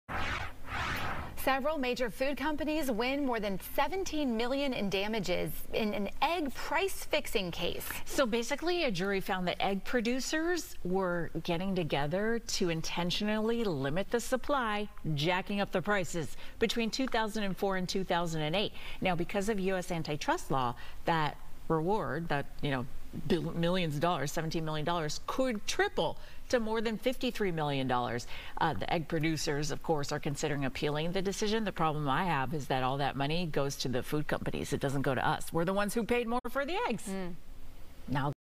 Major food companies awarded over $17 million in damages in egg price-fixing case
Several major food companies have been awarded over $17 million in damages in a price-fixing case against egg producers